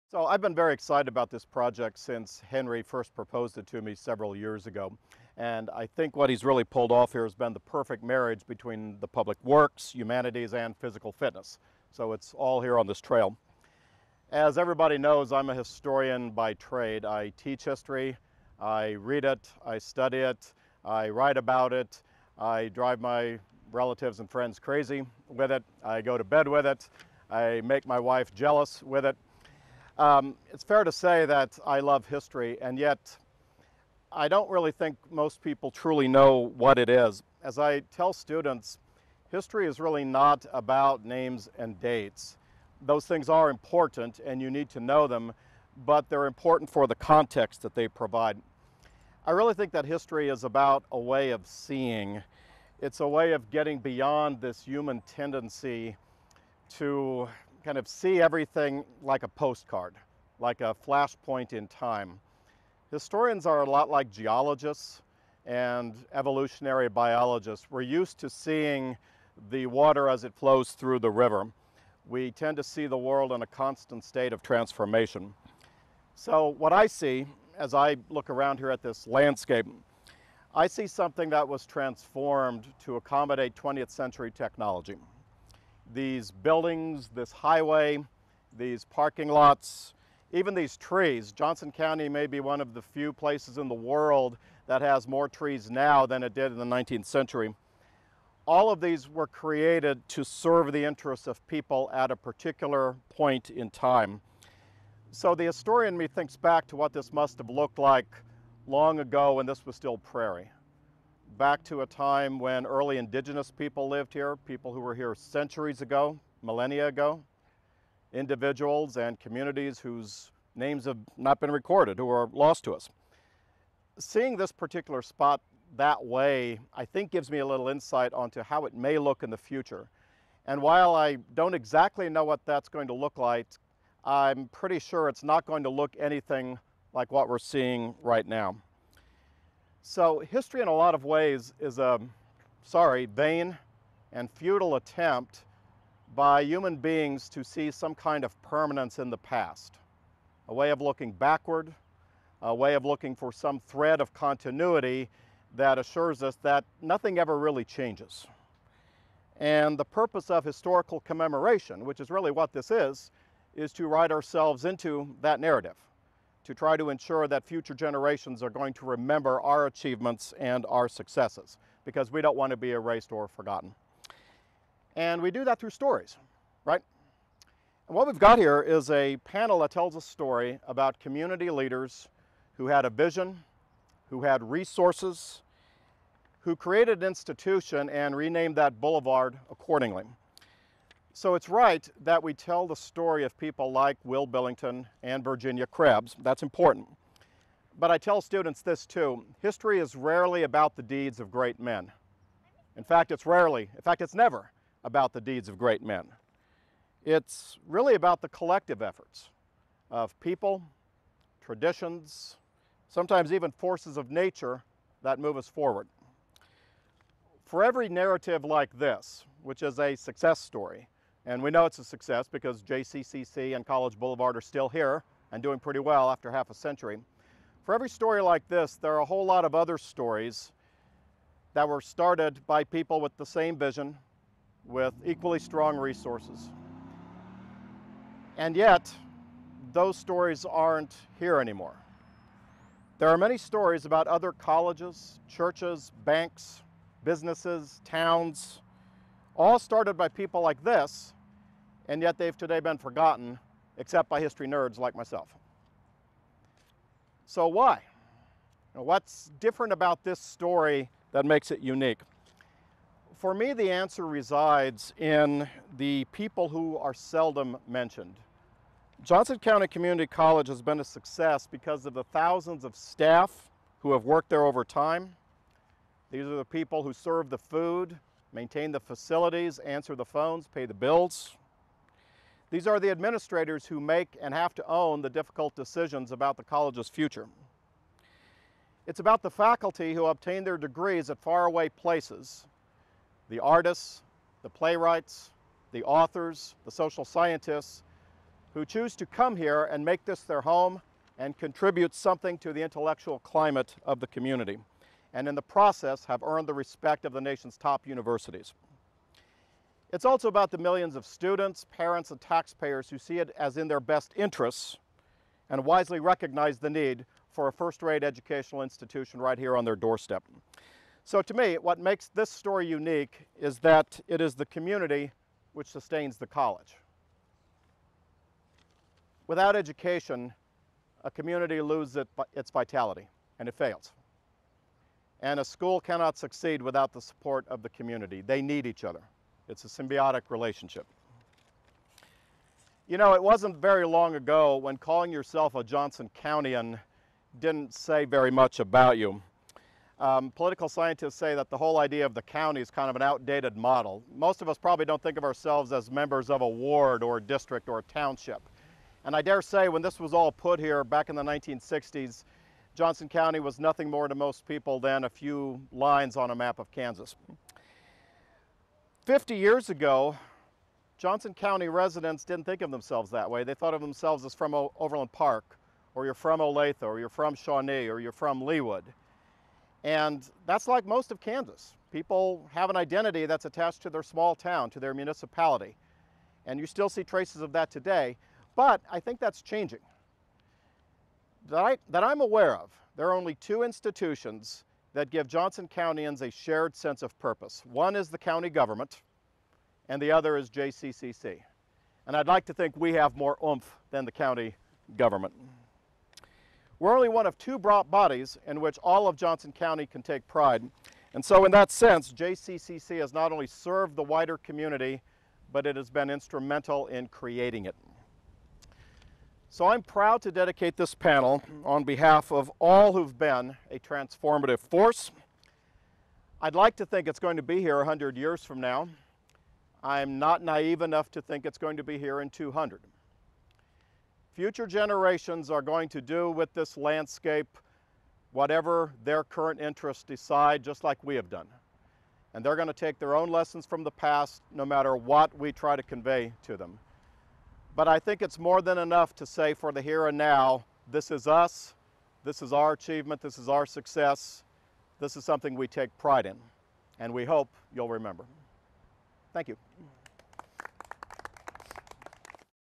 Dr. Jim Leiker, JCCC Professor – Indian Creek Trail Signage
The history of Johnson County Community College has now been shared with the community as part of a major enhancement to the Overland Park segment of the Indian Creek Bike and Hike Trail. The 10-panel installation was a joint partnership by JCCC, area historical societies and the City of Overland Park.

For more information on this and other happenings at the college,